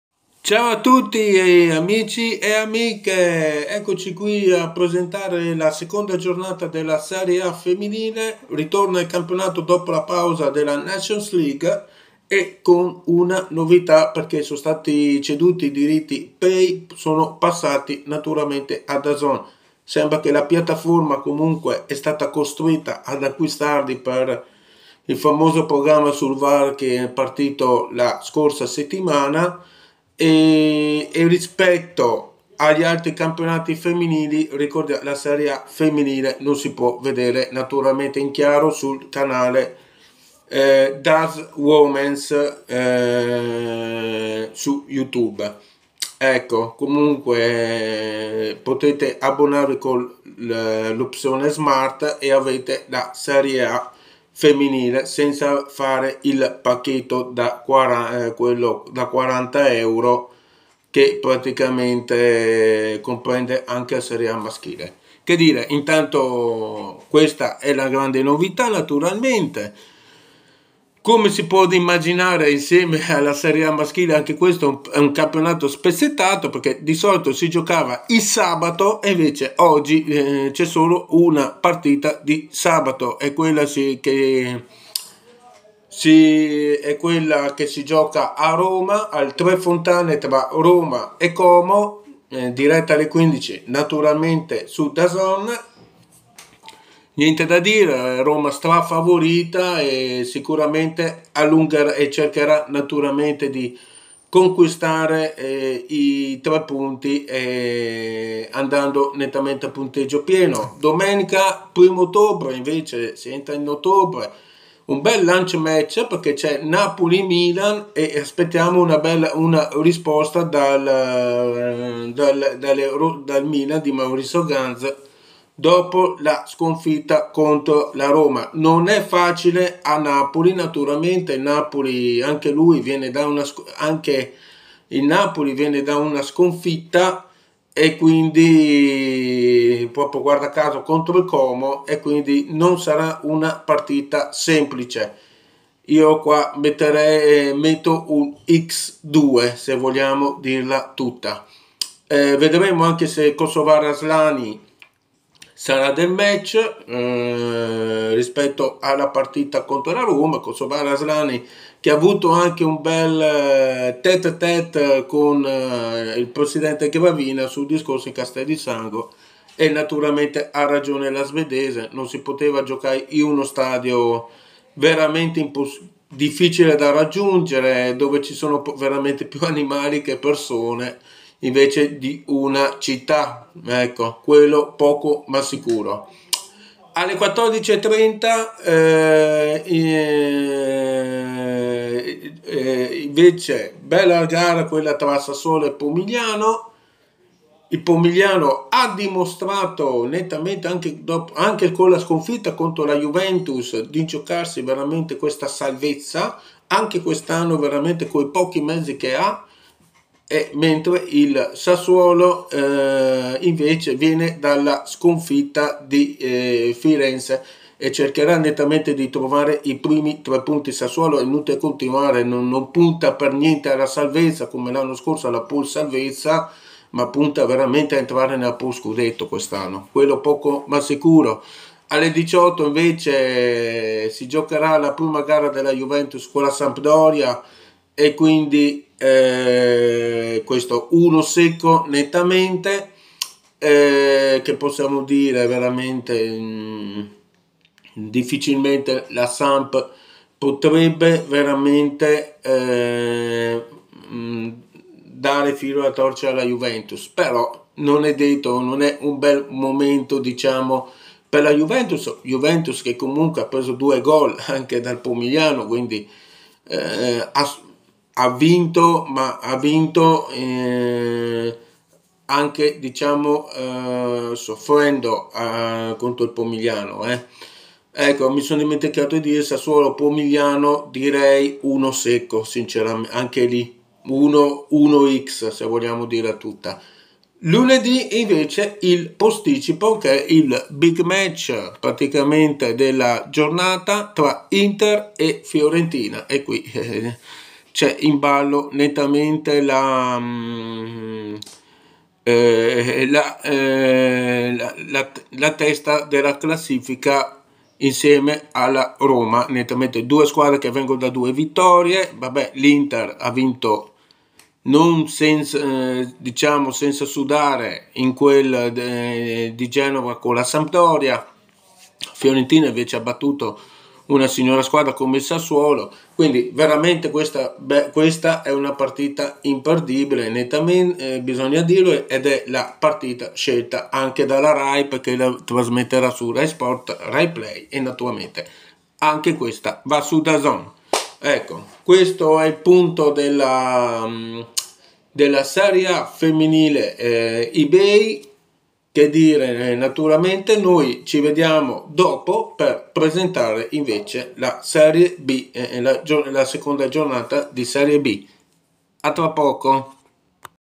Serie A Femminile: presentazione seconda giornata
Serie A Femminile E-Bay 2^ giornata
Oggi
15.00 Roma 🆚 Como [DAZN]
Domani
12.30 Napoli 🆚Milan [DAZN]
14.30 Sassuolo 🆚 Pomigliano [DAZN]
18.00 Juventus 🆚 Sampdoria [DAZN]
Lunedì
18.00 Inter 🆚 Fiorentina [RaiSport, RaiPlay, DAZN]